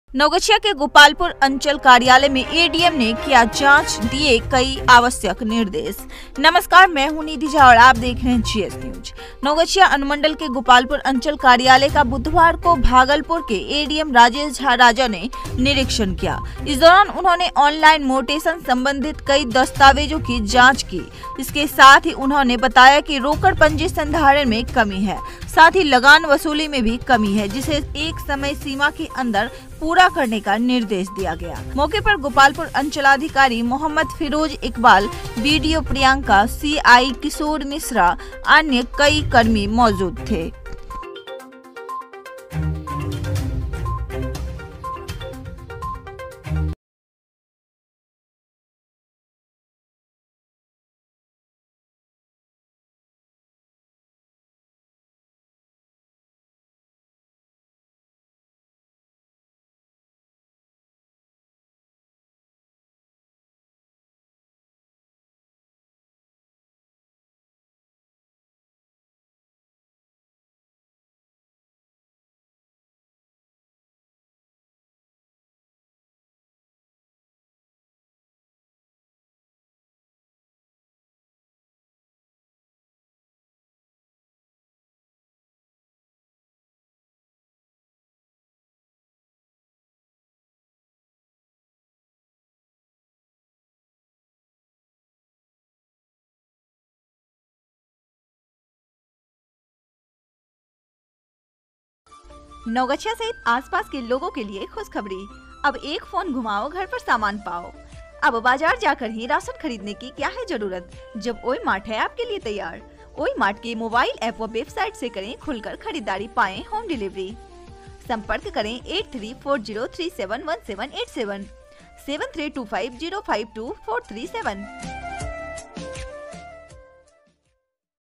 VIDEO : नवगछिया के गोपालपुर अंचल कार्यालय में एडीएम ने किया जांच, दिए कई आवश्यक निर्देश
नवगछिया के गोपालपुर अंचल कार्यालय में एडीएम ने किया जांच दिए कई आवश्यक निर्देश

नवगछिया अनुमंडल के गोपालपुर अंचल कार्यालय का बुधवार को भागलपुर के एडीएम राजेश झा राजा ने निरीक्षण किया ।

 इस दौरान उन्होंने , ऑनलाइन मोटेशन ,संबंधित कई दस्तावेजों की जांच के बाद कई निर्देश दिए ।उन्होंने बताया कि रोकड़ पंजी संधारण में कमी है। साथी लगान वसूली में भी कमी है जिसे एक समय सीमा के अंदर पूरा करने का निर्देश दिया गया है।
 मौके पर गोपालपुर अंचलाधिकारी मोहम्मद फिरोज इकबाल ,बीडीओ प्रियंका, सीआई  किशोर मिश्रा  अन्य कई कर्मी मौजूद थे।

Topic
Naugachia news
Bhagalpur news
Kharik news
Makandpur news
Naugachiya Station
Ismailpur news
Gopalpur news
Rangra news